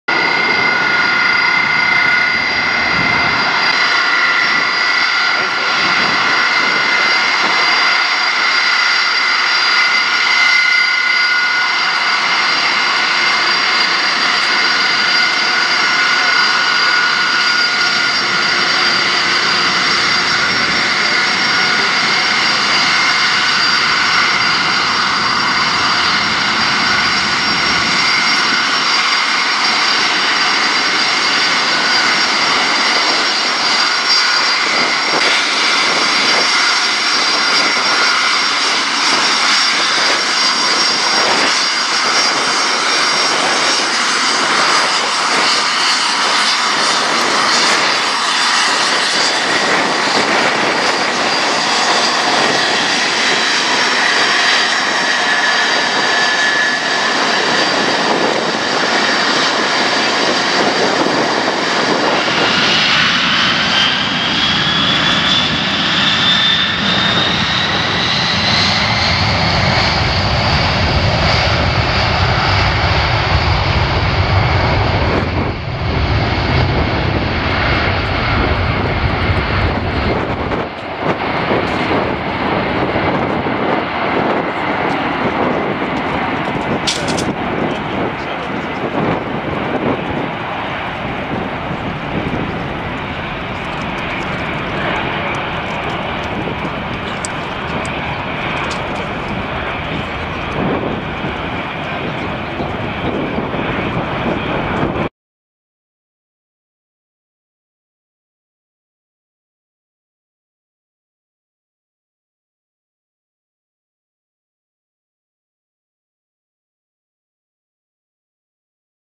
✈[HD] SCREAMING B747-SP VIP - Qatar Amiri Flight - Takeoff @ Hamburg Airport
After Maintenance this Beauty takes off at Hamburg's Runway 33. From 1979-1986 this SP flew for PAN-AM as "Clipper Liberty Bell". After this it flew 9 years for United before converting to an VIP aircraft.